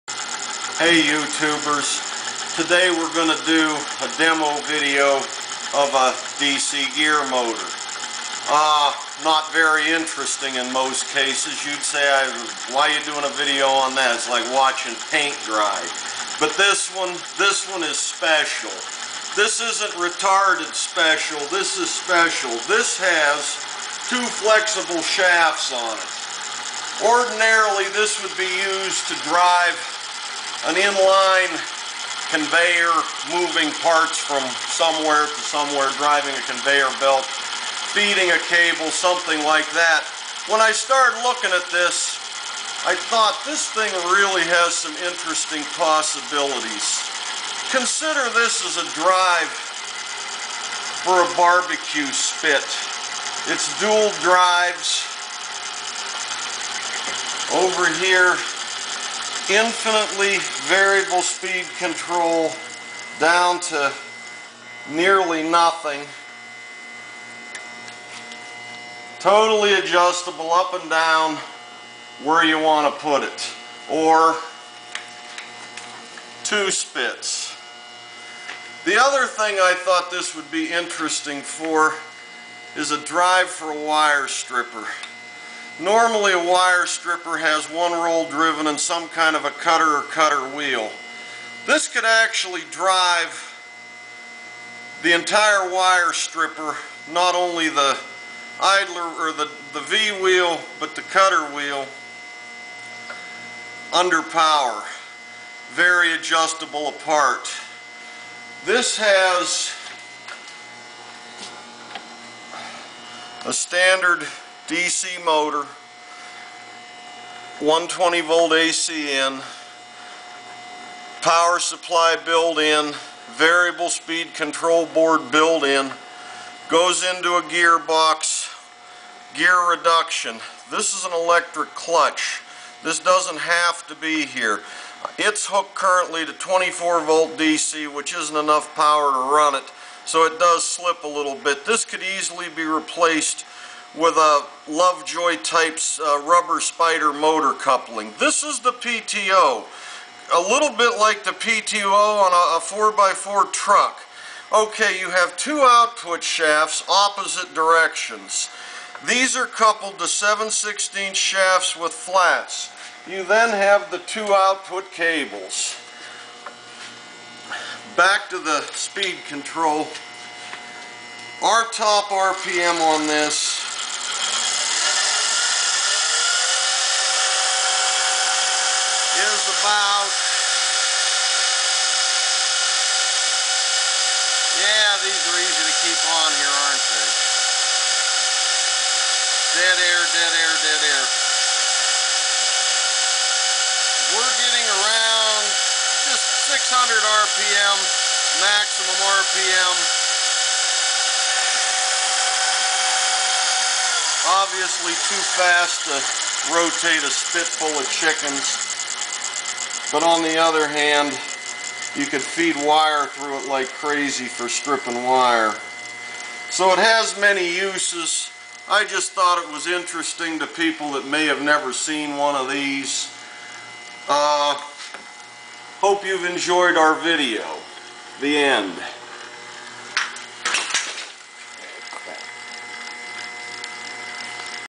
Magnapak Dual Flex Shaft DC Gearmotor.MPG
Short informational video of possible uses for a variable speed, DC gearmotor with power output via 2 flexible drive shafts. Tom Hanks ?